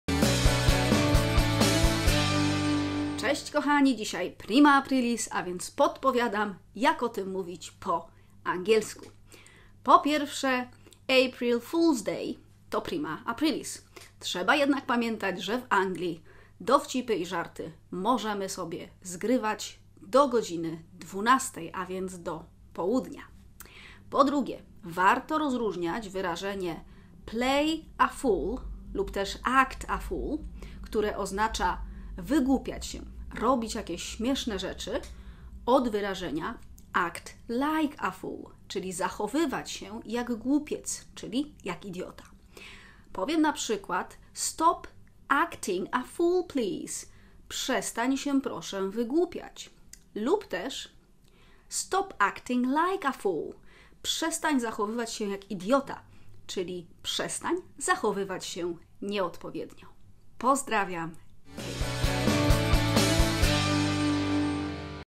➡️ ACT LIKE A FOOL czy ACT A FOOL🤔? jak ich używać po 🇬🇧?
➡️ Dziś primaapris więc warto wiedzieć jak poprawnie używać tych zwrotów 😂😂😂

✅ACT A FOOL - wygłupiać się🤪
✅ACT LIKE A FOOL - zachowywać się jak głupiec 🤬

Proste? Daj ❤️ w komentarzu 😉